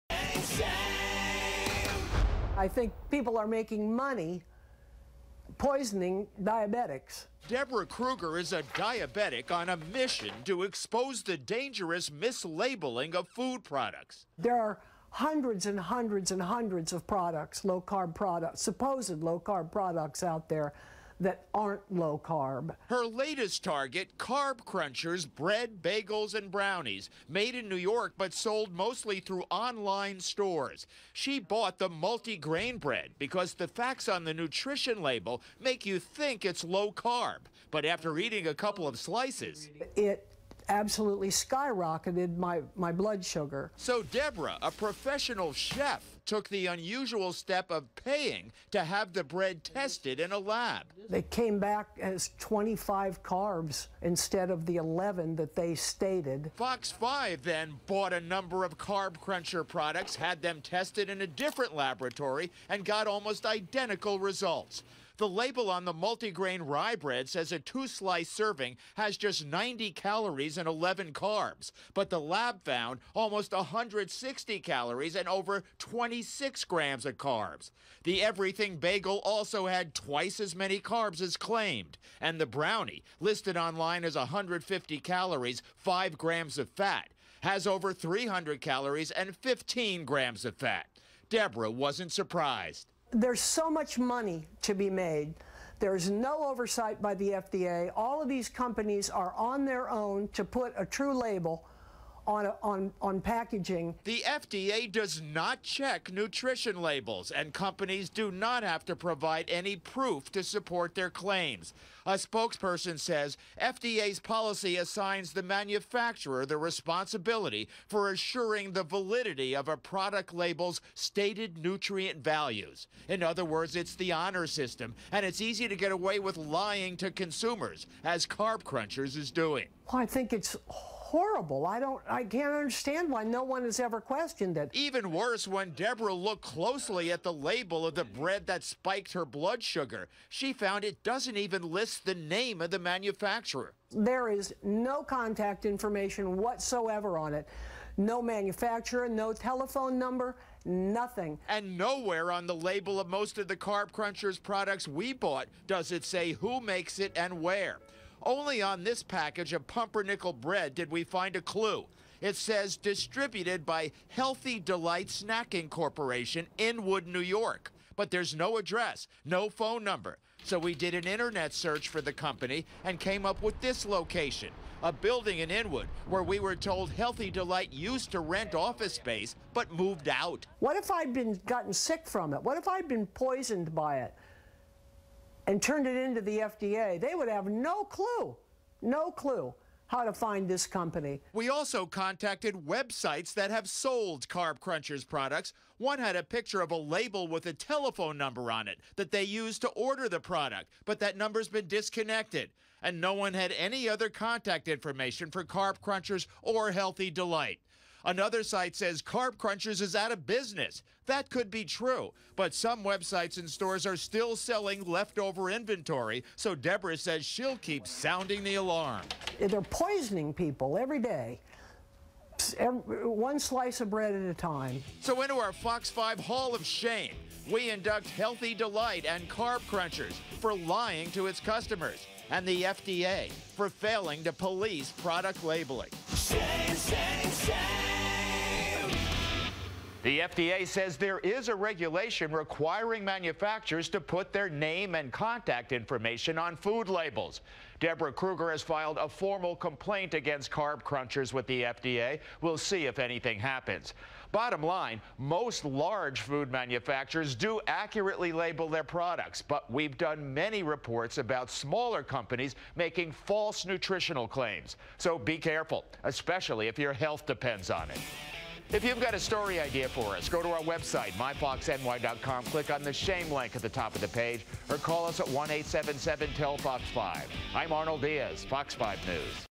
Carb Krunchers Products are Mislabeled!
Carb Krunchers is a company that sells low carb products. However, the nutritional information on the labels are misleading. They are much higher than what's claimed!